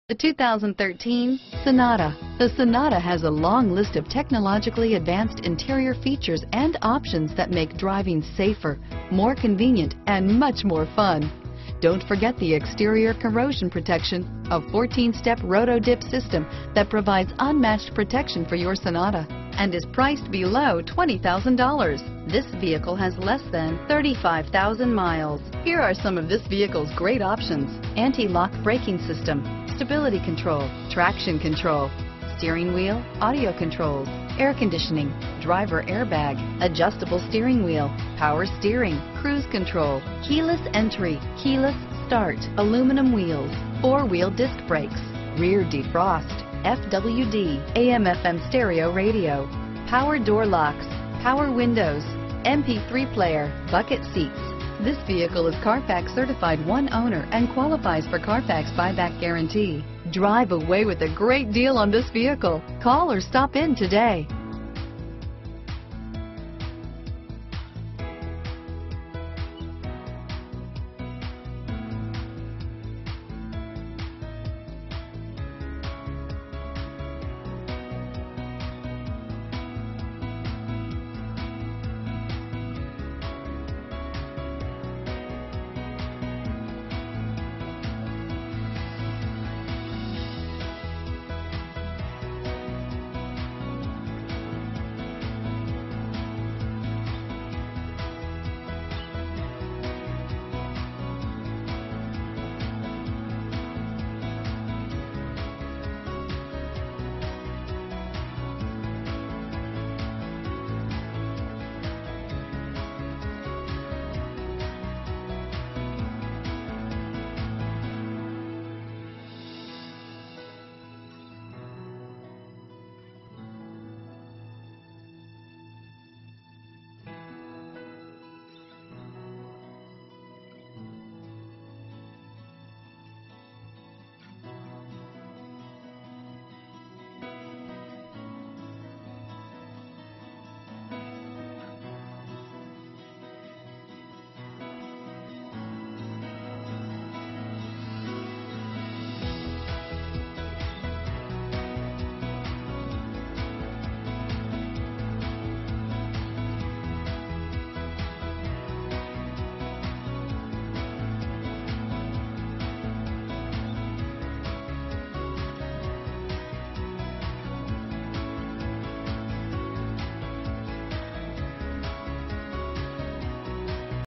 2013 Hyundai Sonata Kennesaw, Canton, Woodstock, Marietta, Alpharetta GA H6719A
Cherokee Hyundai: Servicing Kennesaw, GA near Kennesaw, Canton, Woodstock, Marietta, Alpharetta GA
2013 Hyundai Sonata SE - Stock#: H6719A - VIN#: 5NPEC4AC4DH510292

Cherokee Hyundai
2255 Barrett Lakes Blvd

Climb inside the 2013 Hyundai Sonata! A great car and a great value! This 4 door, 5 passenger sedan just recently passed the 30,000 mile mark! Hyundai infused the interior with top shelf amenities, such as: a trip computer, cruise control, and remote keyless entry. It features an automatic transmission, front-wheel drive, and a 2.4 liter 4 cylinder engine. You will have a pleasant shopping experience that is fun, informative, and never high pressured. Stop by our dealership or give us a call for more information.